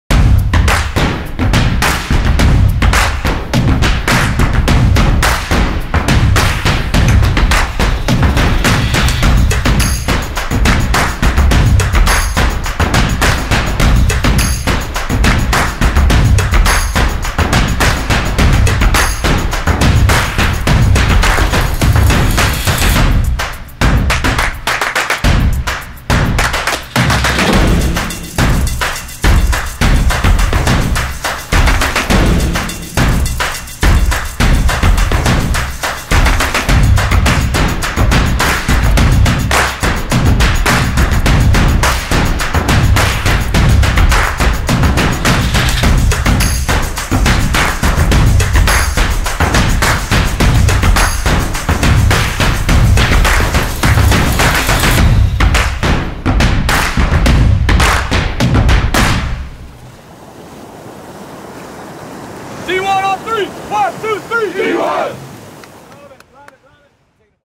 D1 Franchise Summit 2020
Sizzle Reel of the 2020 D1 Franchise Summit in Fort Lauderdale, FL.